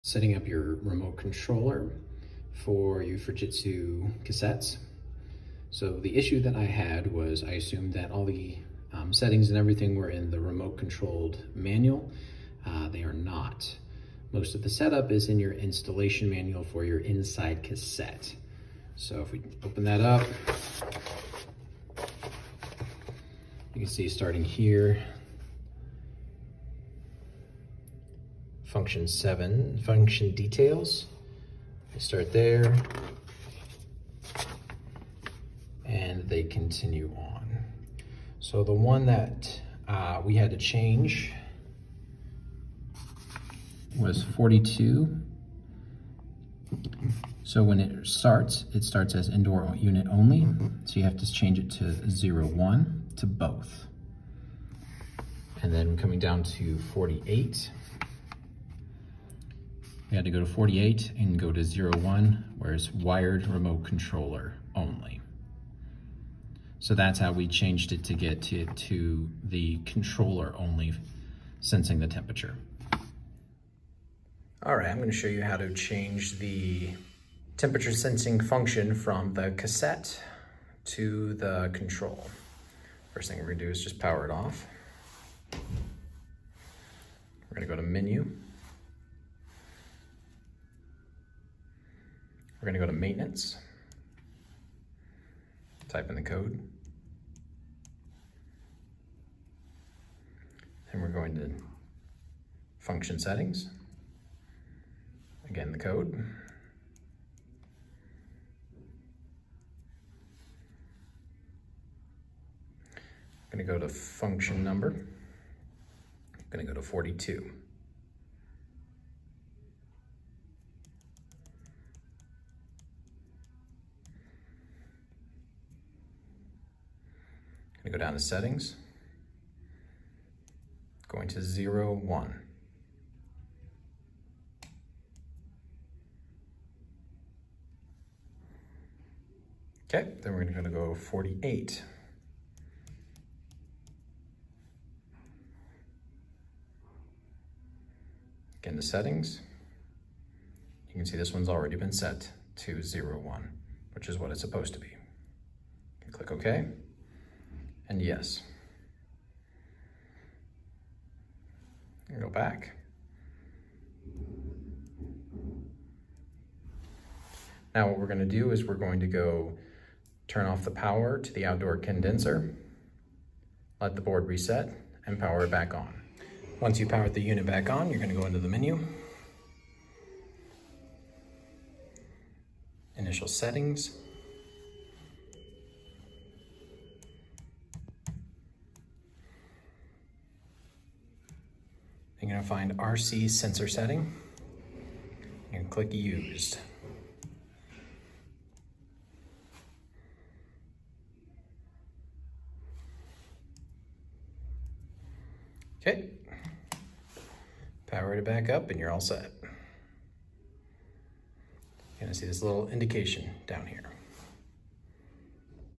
Setting up Fujitsu controller to sense temperature
Setting up the UTR-RNRUZ5 to sense the room temperature . We look at the manual and work on the controller.